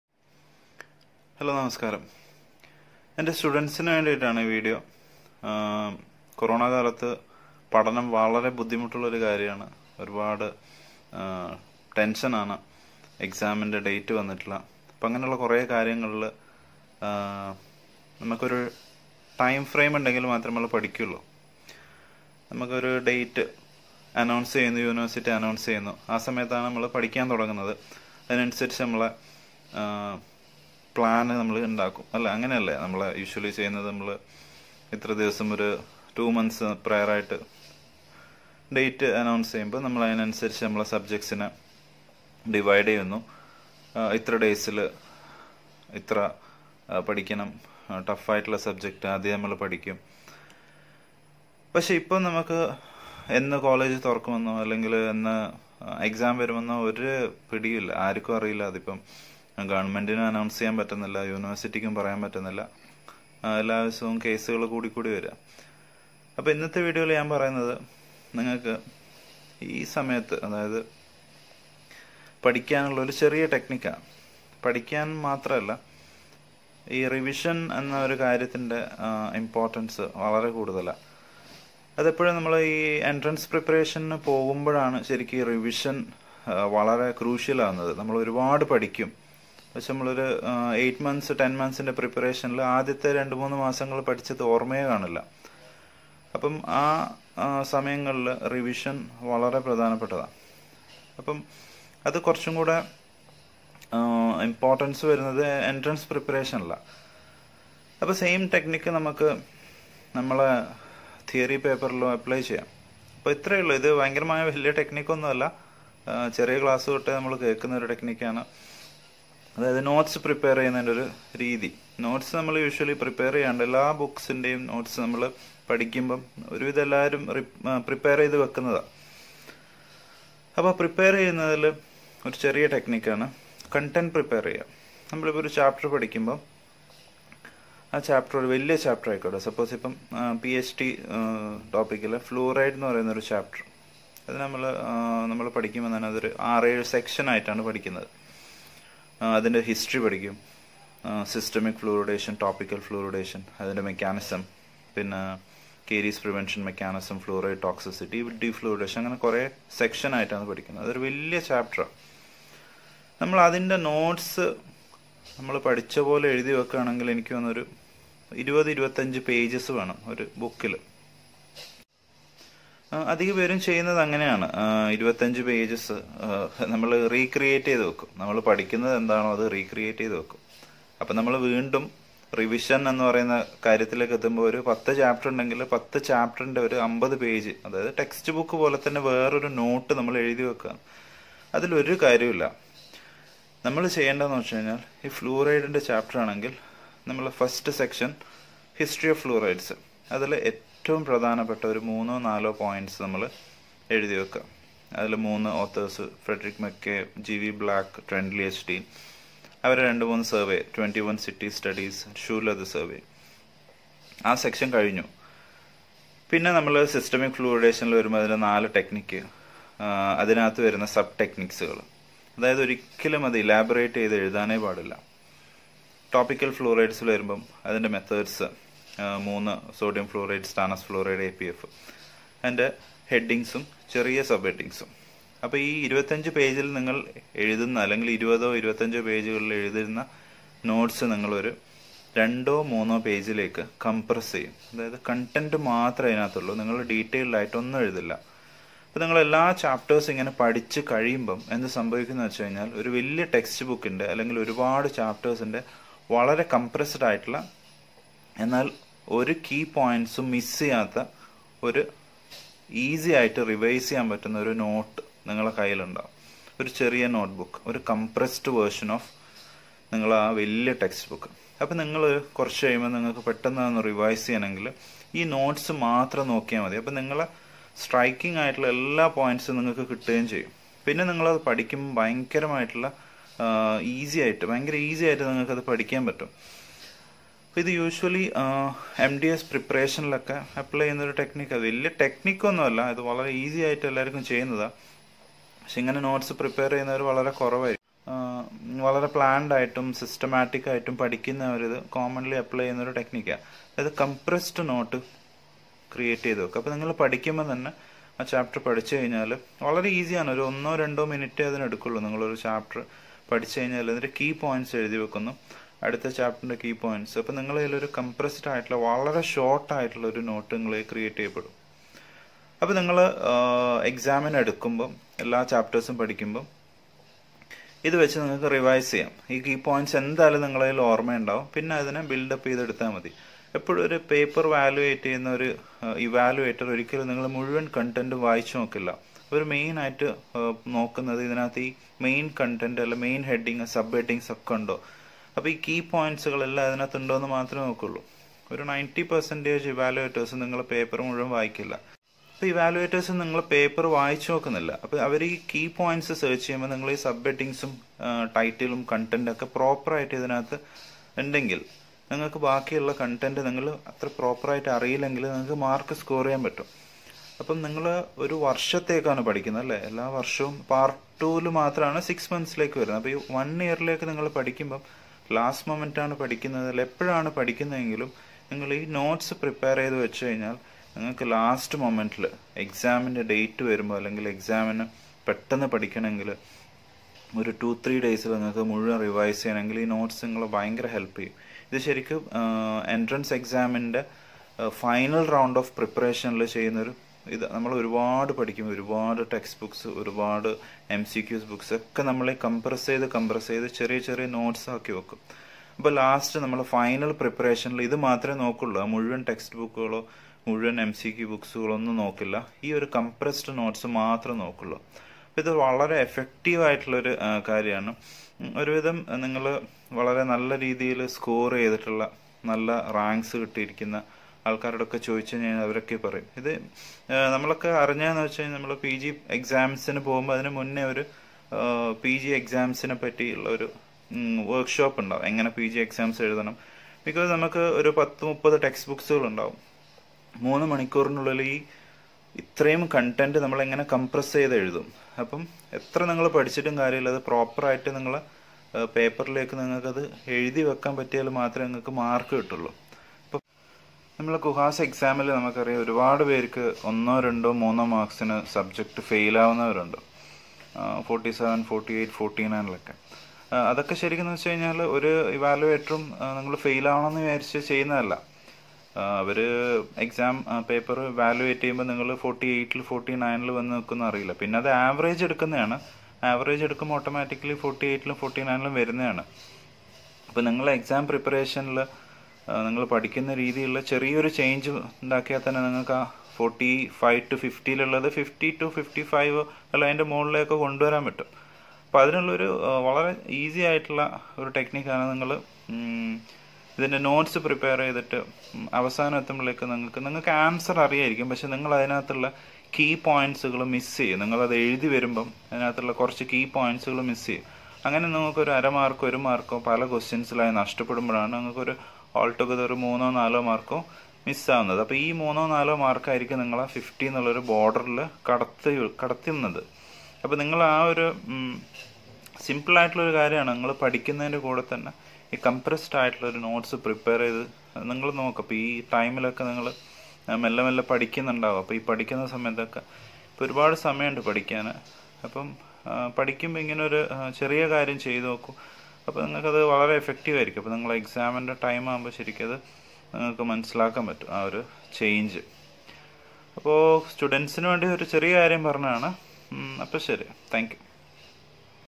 EXAM/NEET PREPARATION -TIPS FOR REVISION(IN MALAYALAM /VLOG 4)
EXAM -REVISION TIPS. HOW TO CREATE COMPRESSED NOTES..

2)Oral medicine & Radilology

3)Oral pathology

4)Pedodontics

5)Orthodontics

6)Public health dentistry